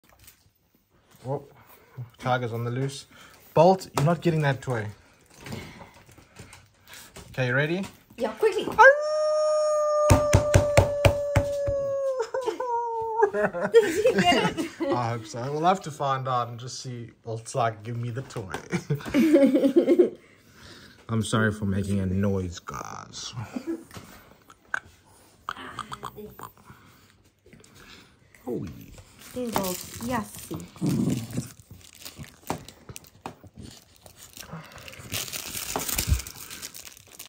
Trying to take photos of 5 French bulldog puppies at once - Day 40
Attempting to take 5 French bulldog puppies at once, while their dad just wants the toy they have 🤣